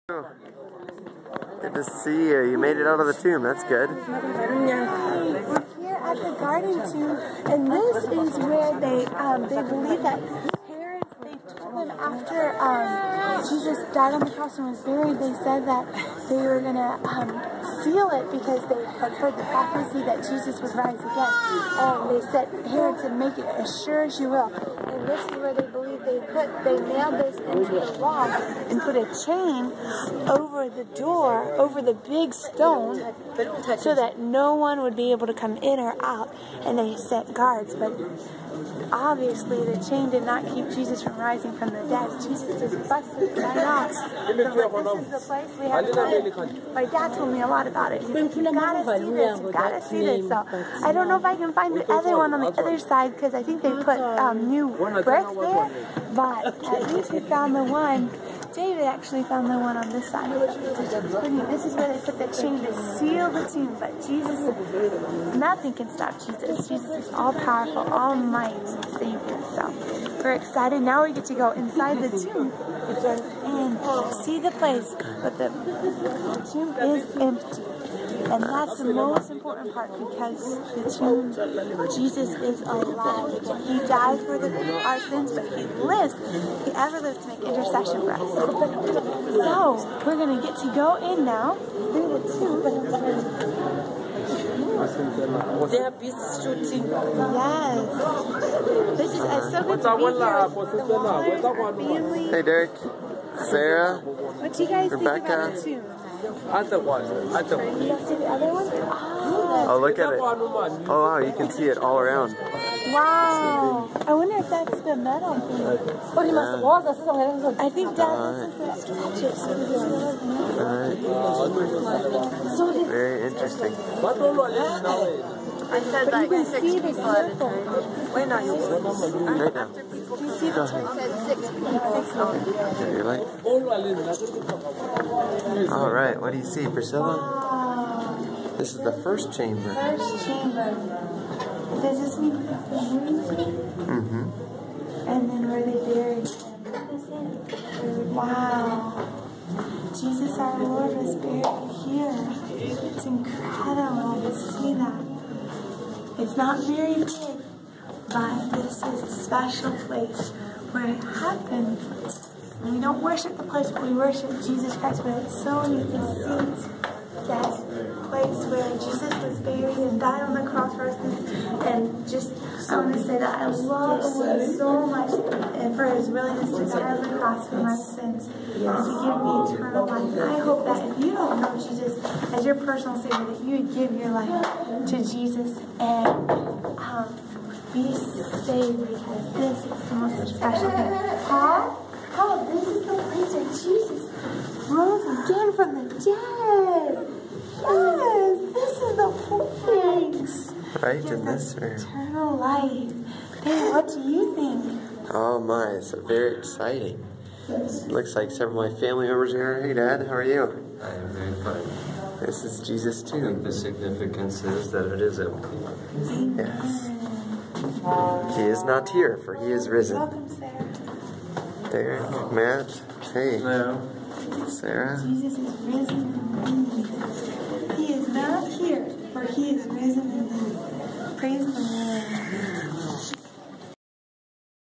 Garden Tomb Part 2
Priscilla shows the empty tomb and explains the significance of the Roman Government sealing the tomb according to the instruction, "Make it as sure as you can."